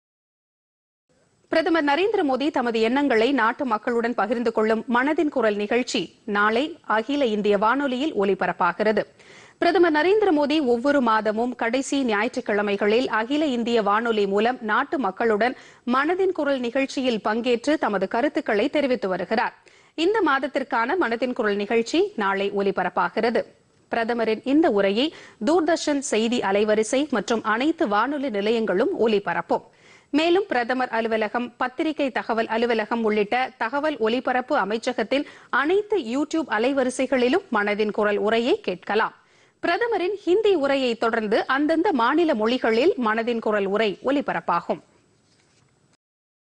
நாளை மனதின் குரல் நிகழ்ச்சிPodhigaiTamilNews
பிரதமர் நரேந்திரமோதி தமது எண்ணங்களை நாட்டு மக்களுடன் பகிர்ந்து கொள்ளும்  மனதின் குரல் நிகழ்ச்சி - அகில இந்திய வானொலி, தூர்தர்ஷனில் நாளை ஒலிபரப்பு.